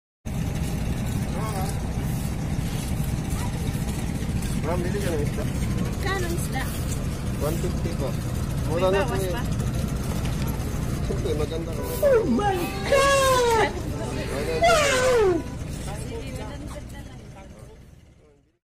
naglako ako ng Isda, may magandang chicks bumili ☺
bili na kayo ng Isda fresh from the sea